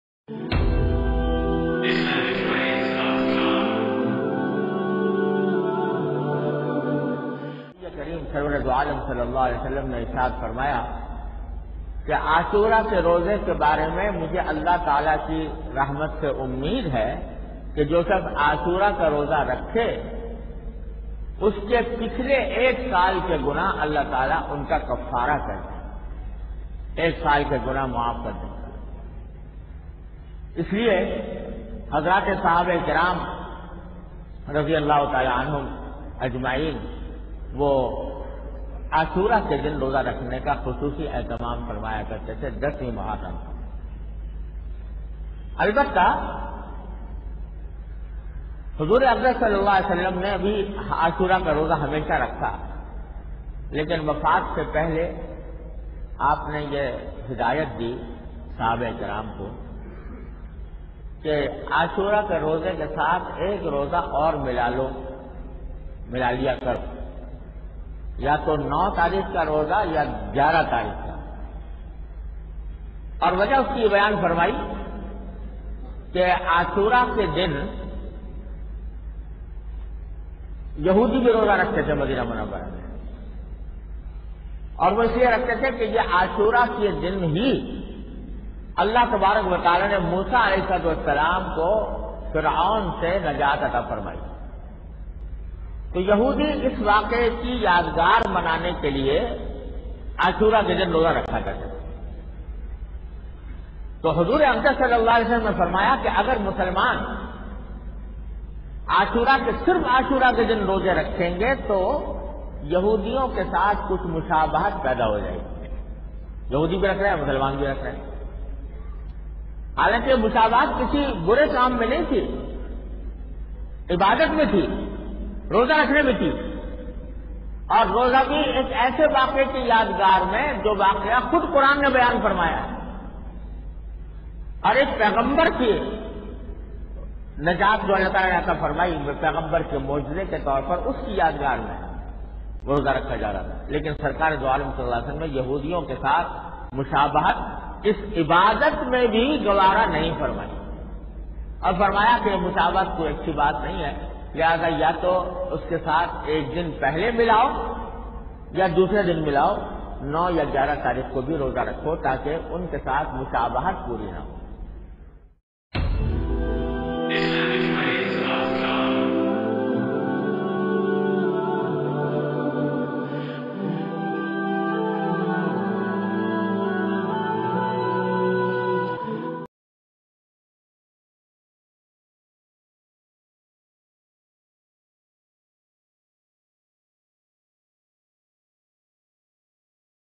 Ashura Ka Roza||10 Muharram Fazeelat
9 and 10 muharram ka roza rakhna kia jaiz hai muharram ka roza ki fazilat watch in this video dtail